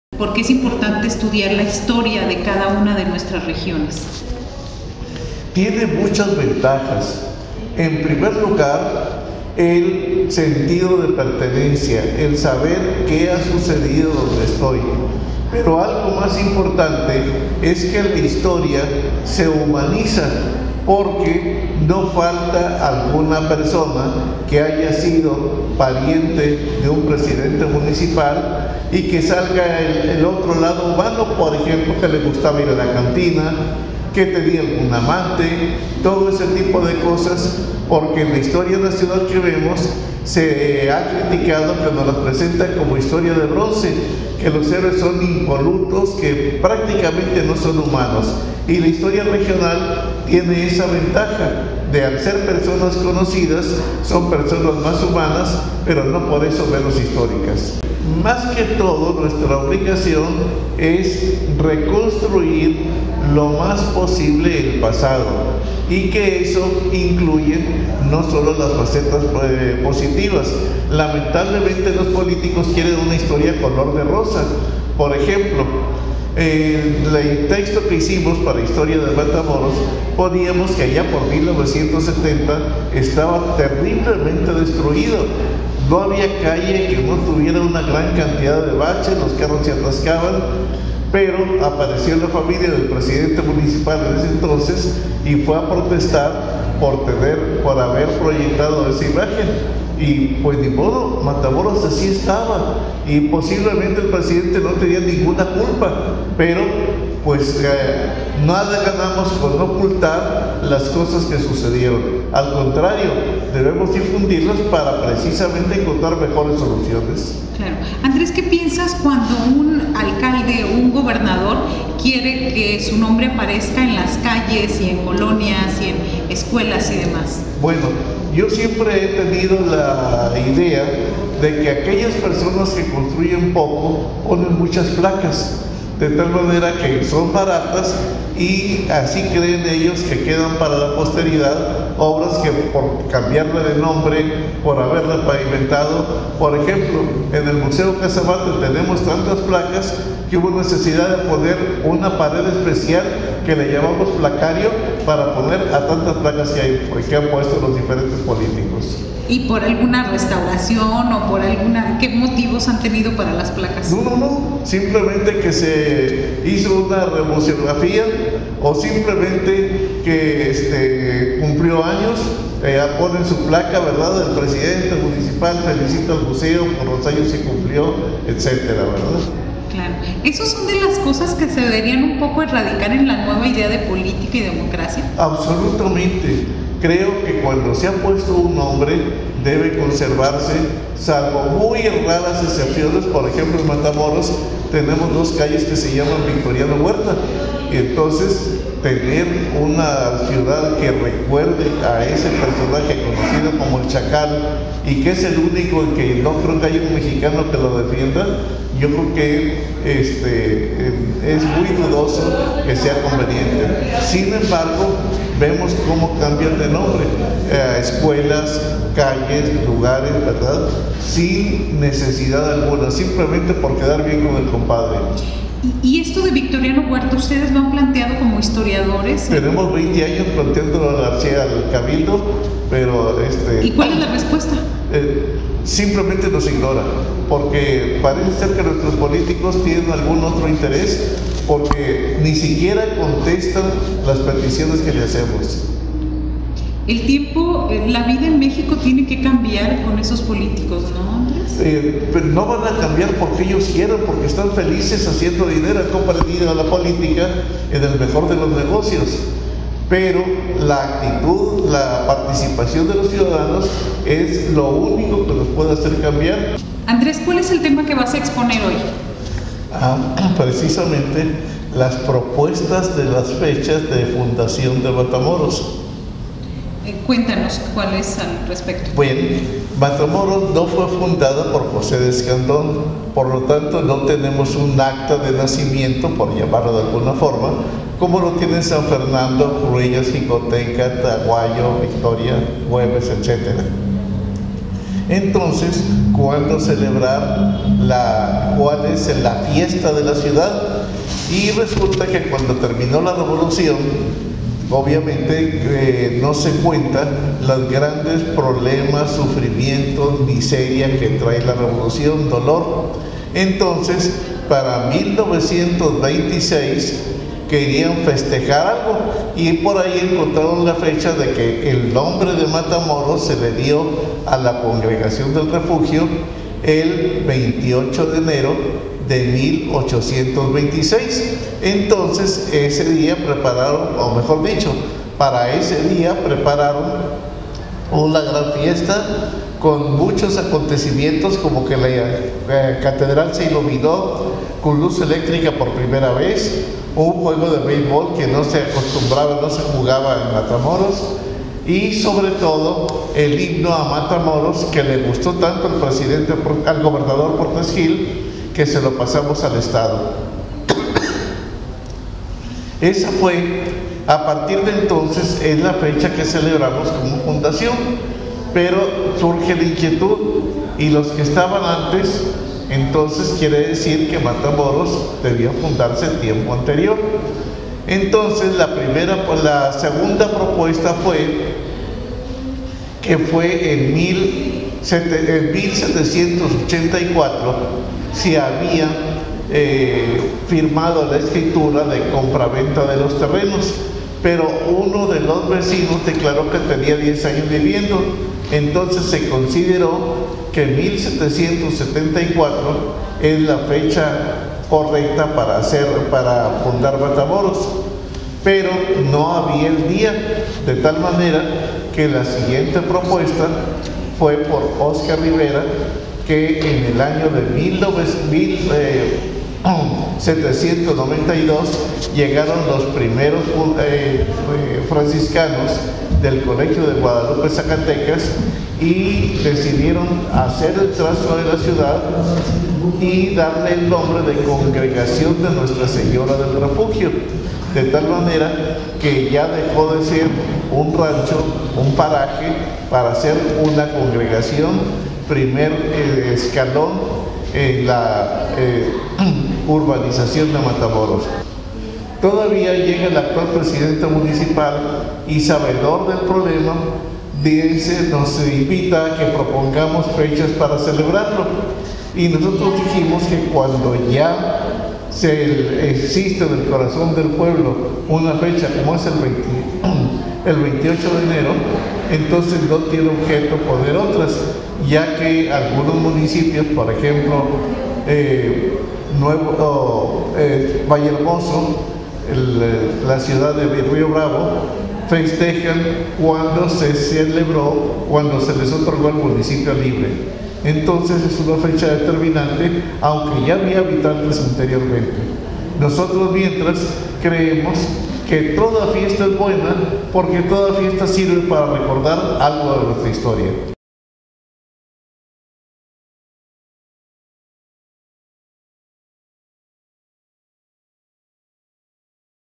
Crónicas de Matamoros, con Andrés Cuéllar
Victoriano Huerta, el traidor de la Revolución Mexicana, es el nombre que tienen dos calles en Matamoros Tamaulipas y desde hace más de 20 años, historiadores y estudiosos, encabezados por el Cronista Andres Cuellar Cuellar han pedido a los alcaldes en turno el cambio y NO han recibido respuesta.

Hoy en La Entrevista el maestro nos habla de este y otros temas En Un 2x3 Tamaulipas.